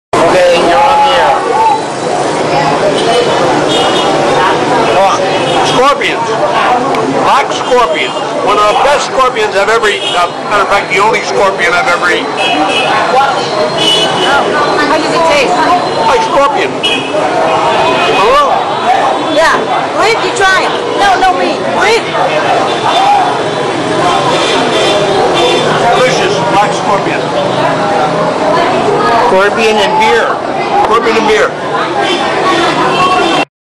Eating Scorpions in Hanoi Vietnam
At a Bia Hoi Restaurant in Hanoi..
I took some friends to try fresh beer and
Scorpions..